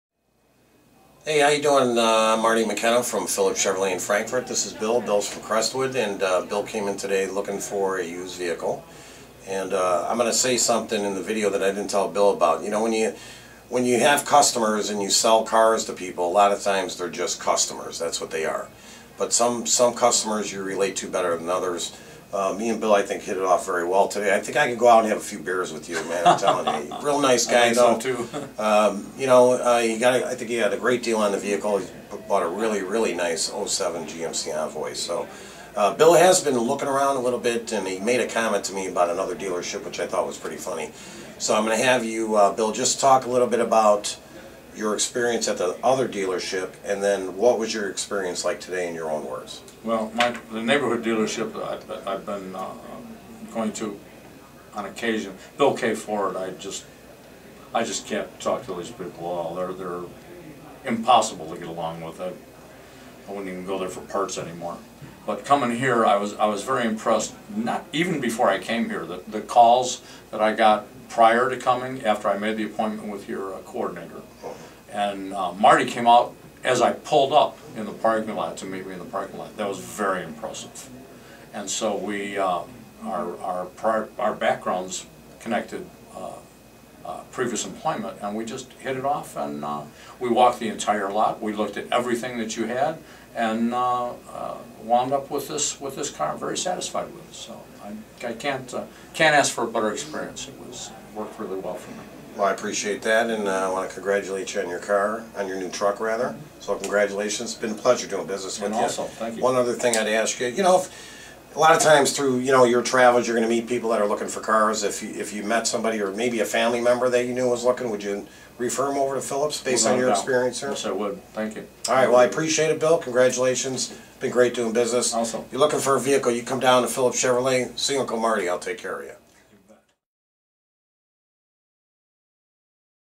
2007 GMC Envoy - Customer Review Phillips Chevrolet - Used Car Dealer Sales Chicago
- Here at Phillips Chevrolet in Frankfort, IL customers not only enjoy the great deals they get on new and used cars, but our sales consultants' personalized attention. Bill is here with sales consultant, Marty McKenna and Bill just purchased a 2007 GMC Envoy and he couldn't be happier with his experience. When Marty asked Bill about his experience Bill said, "Before I even came here, I was surprised how much they stay in contact with their customers. Marty was very personable and I'm very satisfied with how everything went! I can't ask for a better experience!" Come down and get the great deals and customer service we have to offer you!